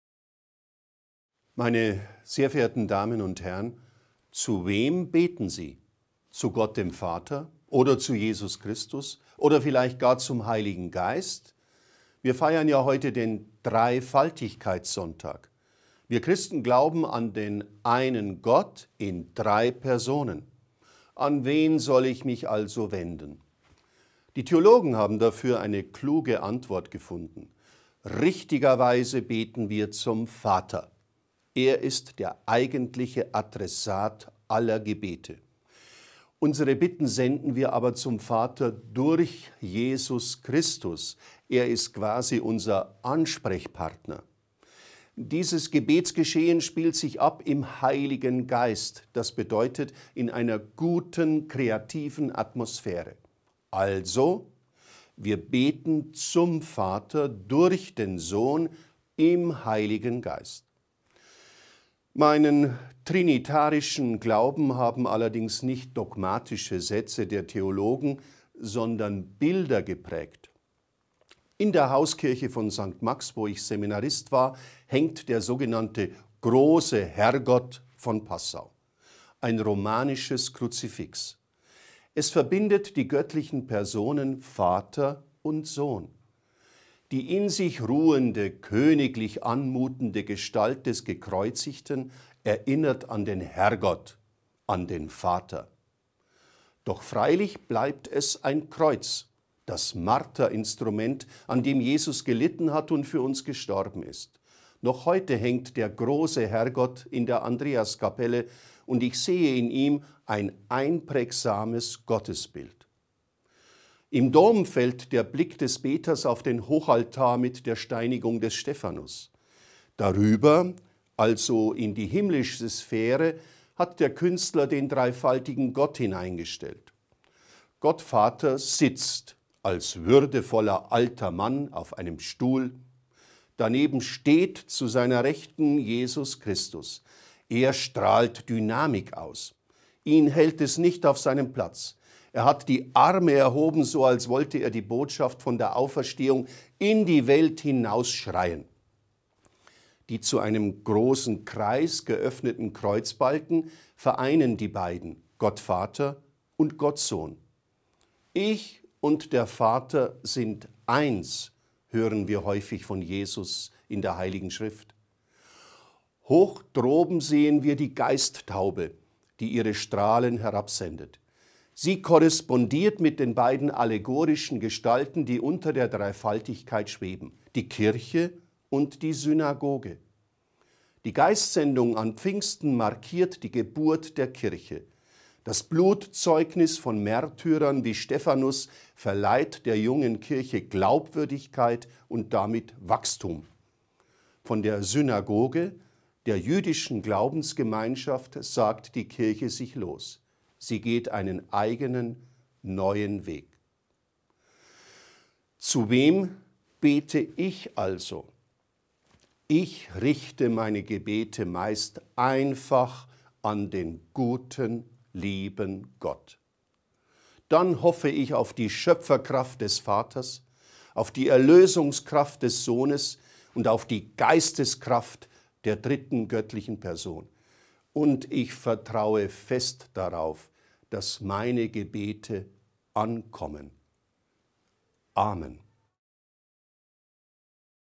Zu wem beten? - Impuls zum 4. Juni 2023
Die Trinität (Dreieinigkeit)  Gottes im christlichen Glauben führt bei dem ein oder anderen Gläubigen oftmals zur Verwirrung. Zu wem soll man denn jetzt eigentlich beten? Eine einfache und bildhafte Antwort darauf gibt Dompropst Dr. Michael Bär in seiner Predigt zum Dreifaltigkeitssonntag am 4. Juni 2023.